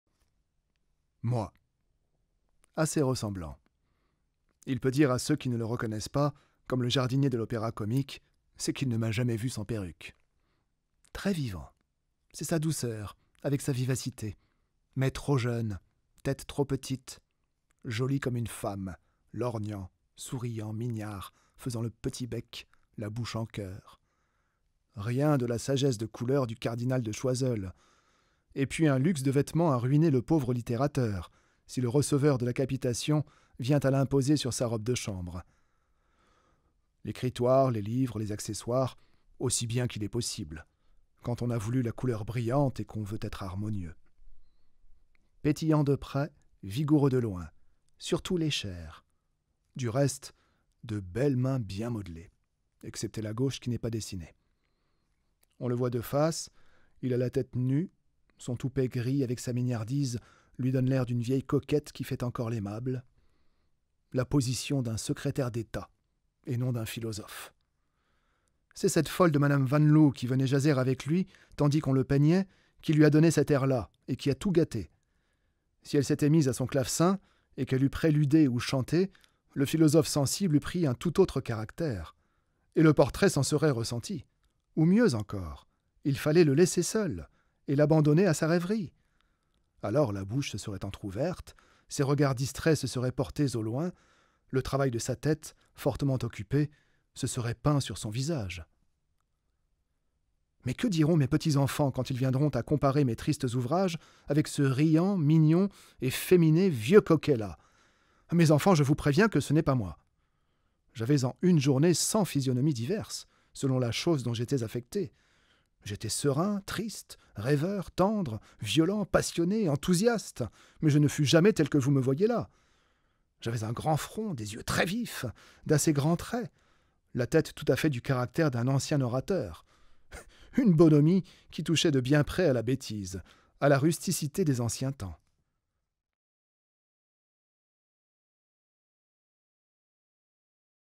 Portrait de Denis Diderot commenté par lui même
Dans ses commentaires sur le Salon de peinture de 1767, exposition organisée tous les deux ans au Musée du Louvre, Denis Diderot nous livre une description de son portrait peint par Michel van Loo.
Interprété par Malo de La Tullaye.  Extrait du spectacle "Flânerie en compagnie de Diderot" de l'Ensemble Artifices.